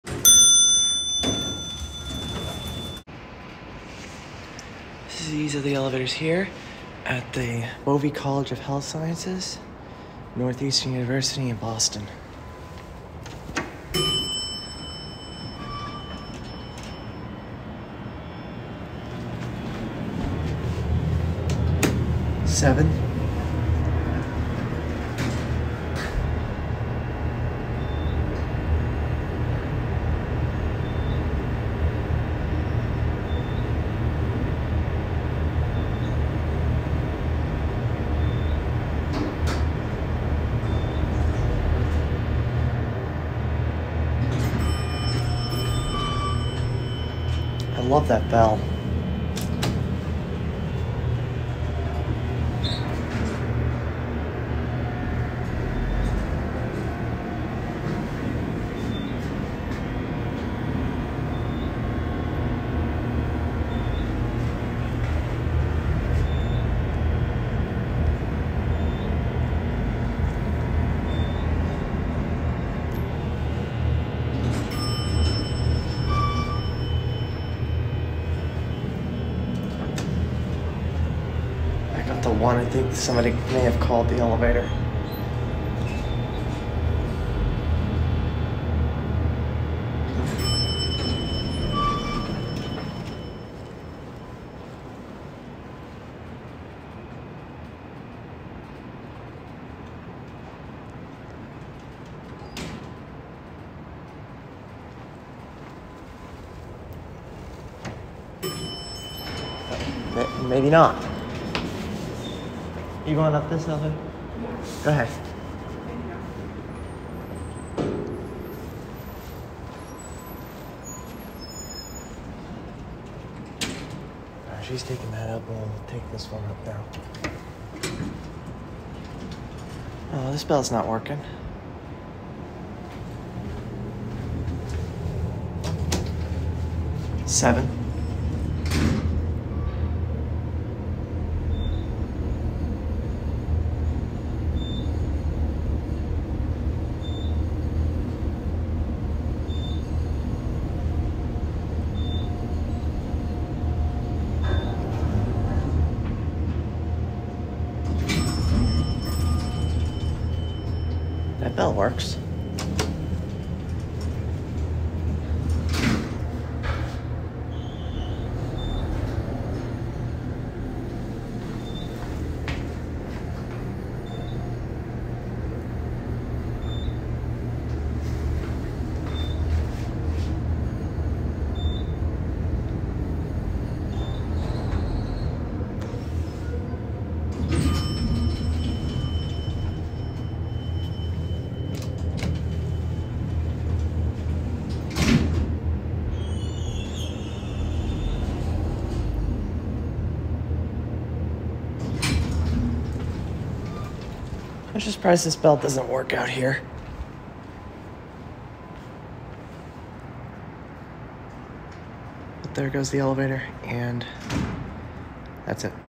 Elevators at Bouvé College of Health Sciences; NU; Boston MA
(11-9-24) Delta Beckwith elevators here and have a neat bell. Love these elevators and there also traction. They run fast and smooth